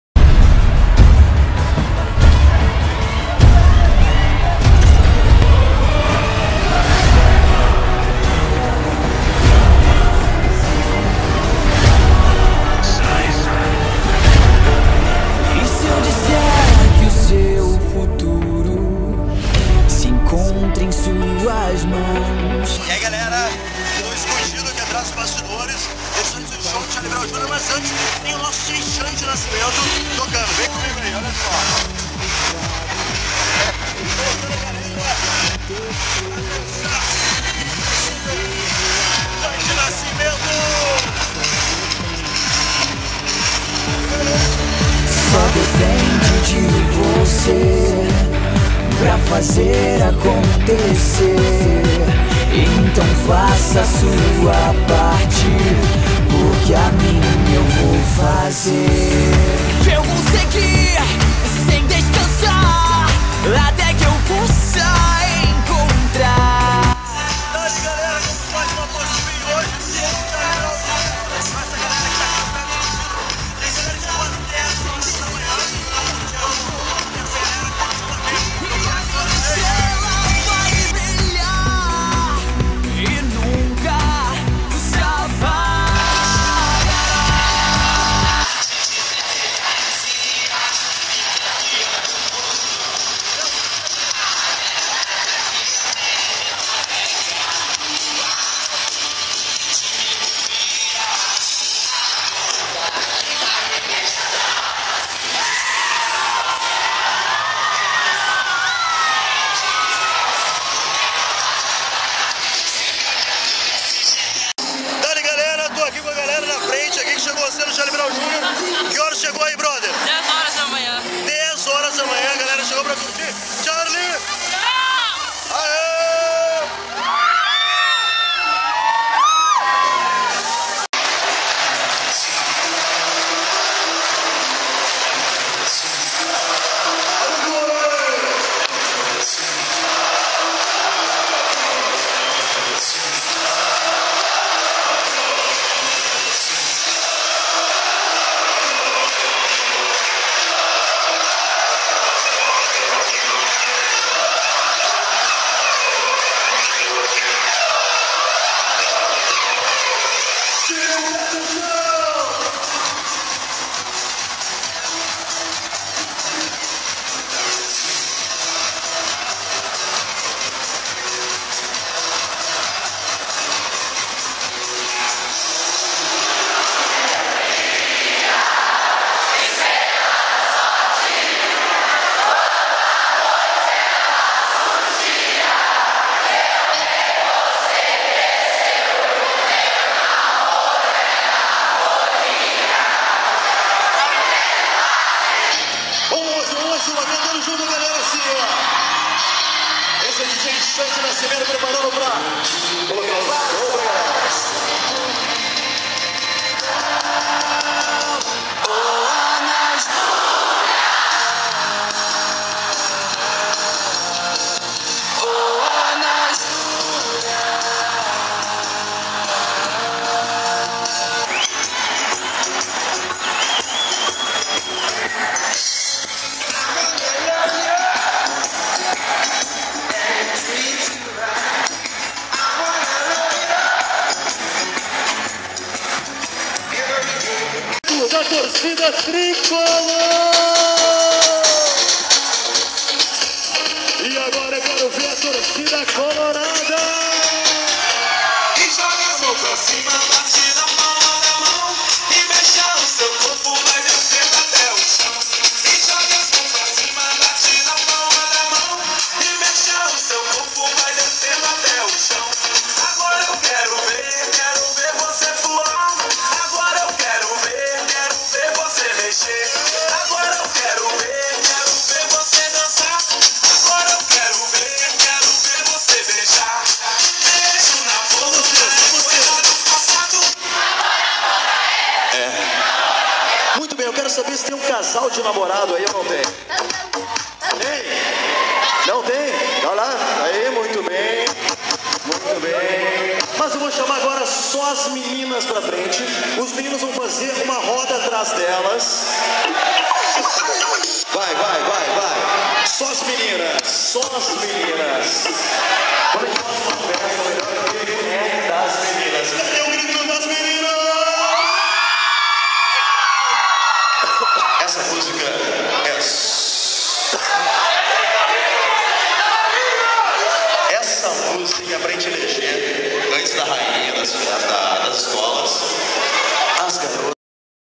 Zeh Antonio e Dj Xandy Nascimento - fazendo o agito no Pepsi
Se liga no o agito que o Xandy e o Zeh fizeram no Pepsi On Stage antes do show do Charlie Brow no dia 20 de outubro de 2012.